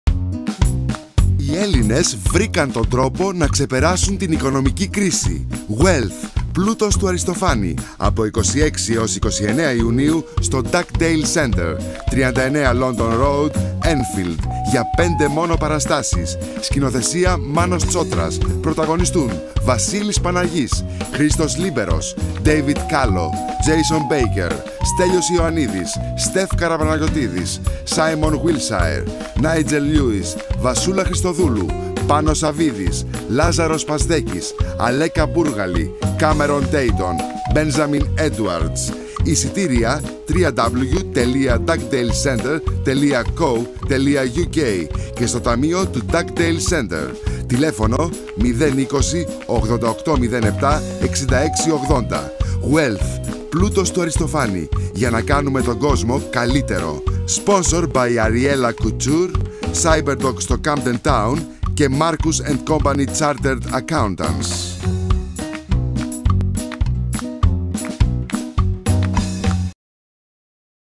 "Wealth" Commercial Spot - ῾Πλούτος῾ διαφημιστικό σποτ 2013
WEALTH by Aristophanes. The Greeks found the way to get out of the economic crisis... Only in Dugdale Centre from 26 to 29 of June 2013... Book your tickets now or call ... Wealth to make the world a better place... a big comedy party!
Director/Producer : Manos Tsotras
Producer: Vasilis Panayis
Crew: Vasilis Panayis, Christos Liberos, David Kallo, Jason Baker, Stelios Ioannidis, Stef Karapanagiotidis, Simon Willshire, Nigel St Lewis, Vasoula Christodoulou, Panos Savvides, Lazaros Pasdekis, Aleka Bourgali, Cameron Deighton, Benjamin Edwards.

Video Production: Maxmasses
Filmed and Edited by Demetra Nikitakis
Composer: Stalo Georgiou